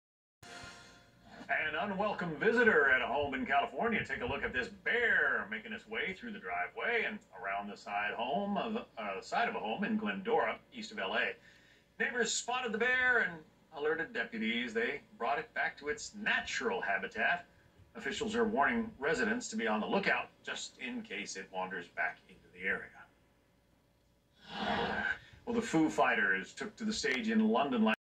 September 4, 2022
bear walking around Glendon